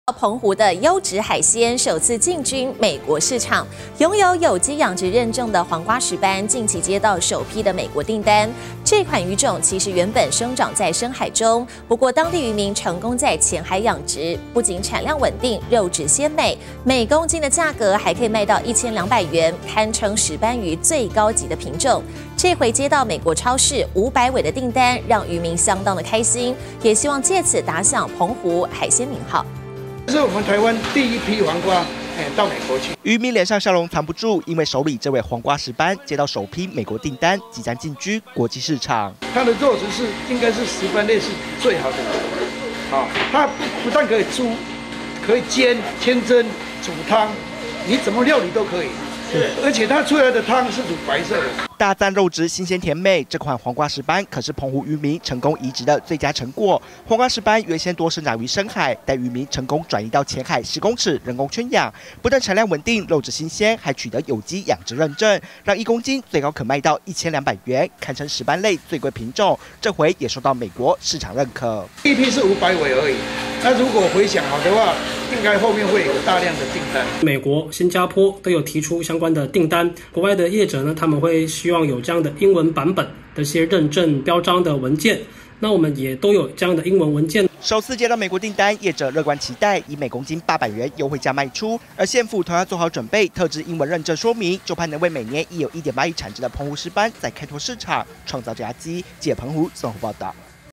成功外銷！ 澎湖「黃瓜石斑」進軍美國超市
澎湖的優質海鮮，首次進軍美國市場！擁有有機養殖認證的黃瓜石斑，近期接到首批美國訂單，這款魚種，其實原先生長於深海中，不過當地漁民成功在淺海養殖，不僅產量穩定、肉質鮮美，每公斤價格還可以賣到1200元，堪稱石斑魚最高級品種。這回接到美國超市500尾的訂單，漁民相當開心，希望藉此打響澎湖海鮮名號。